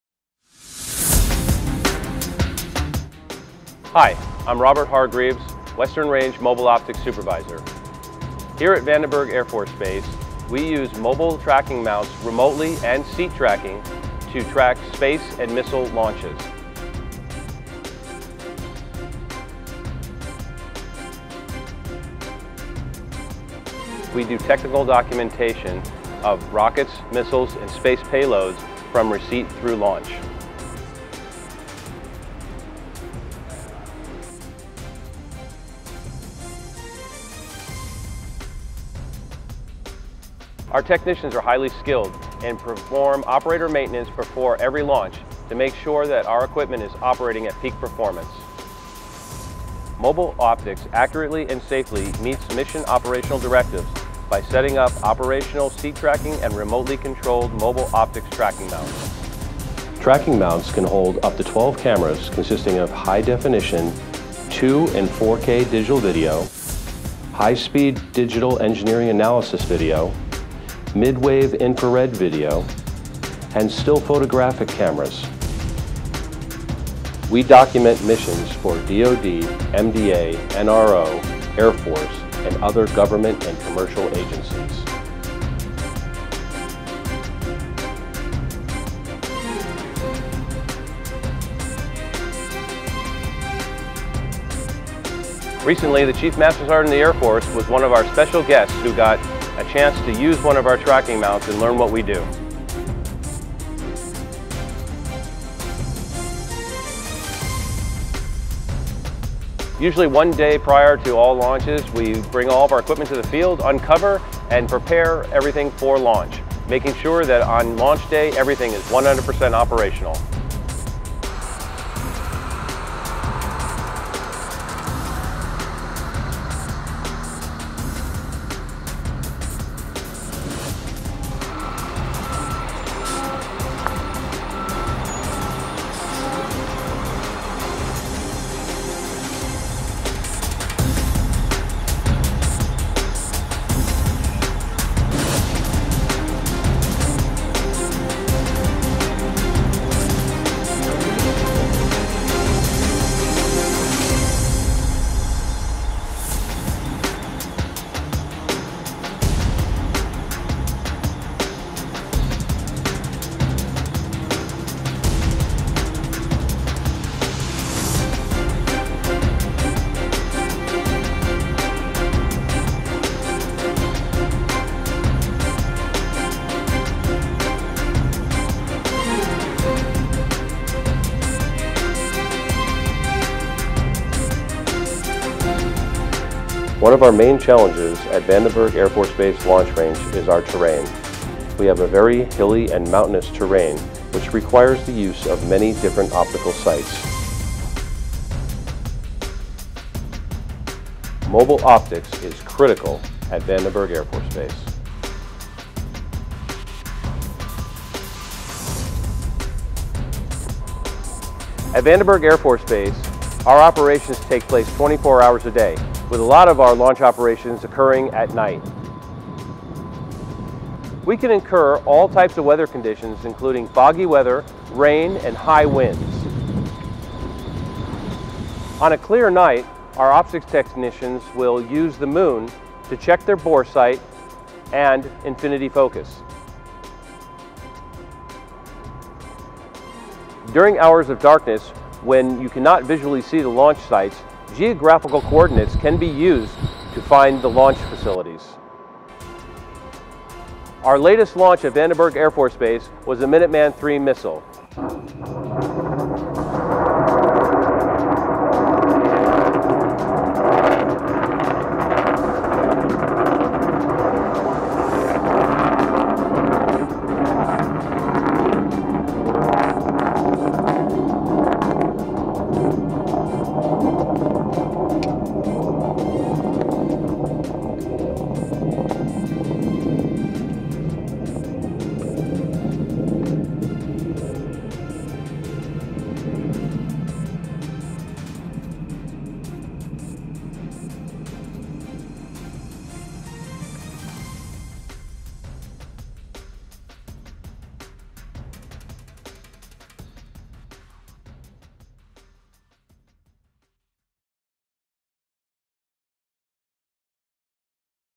30th Communications Squadron Mobile Optics
30th Communications Squadron Optics shop records and documents all launches from Vandenberg Air Force Base. They use tracking mounts with up to 12 cameras. Optics supports their customers with technical documentation from cradle to grave.